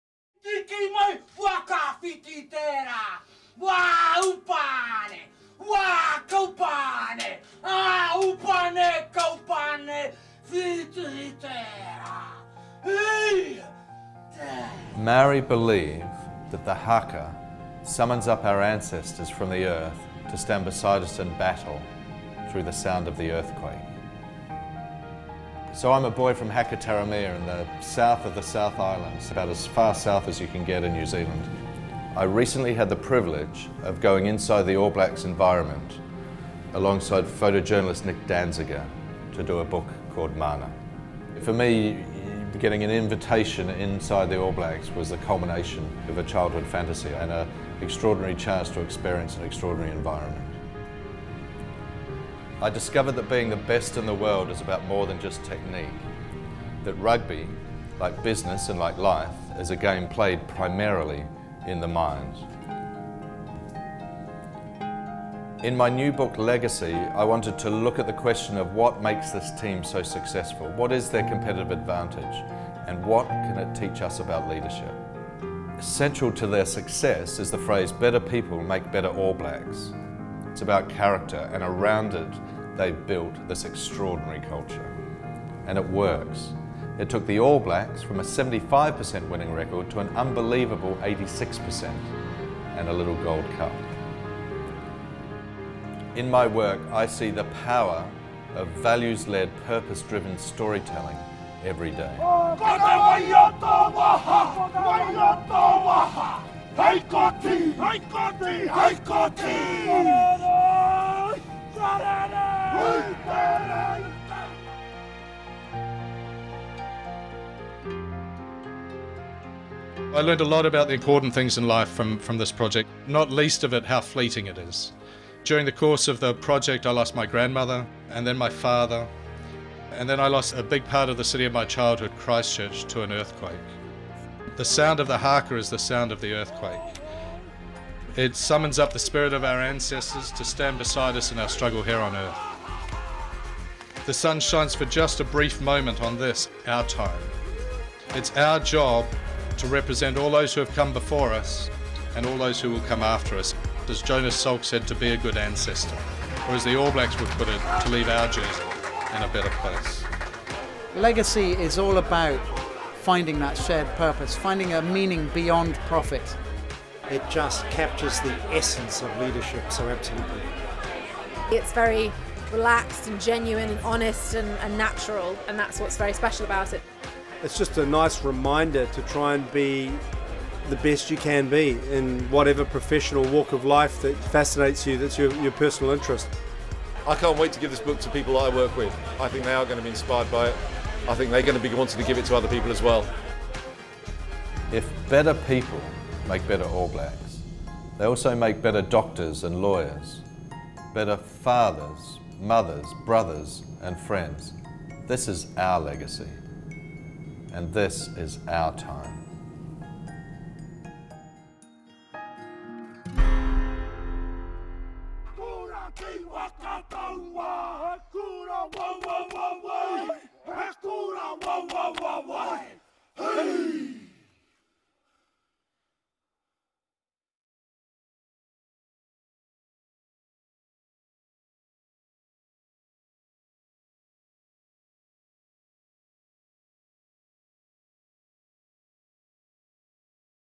Legacy - Director's Cut
A Pink Banana Studios Production - pinkbananastudios.co.uk

In November 2013, James Kerr launched Legacy: What the All Blacks Can Teach us About the Business of Life. 15 Lessons In Leadership, his carefully researched book, which delves into the mentality of the most successful sporting team of all time and finds universal lessons one can learn from them in business, and indeed, in life. We were delighted to put together this short film to launch the book and illustrate some of its key points. We shot the film in the notorious All Blacks black and white and let the narrative be led by an interview with the author. What we love about this film is how it conveys not only James' enthusiasm for his subject matter and business, but his genuine excitement at living out a boyhood dream and enter into the inner workings of All Blacks Rugby.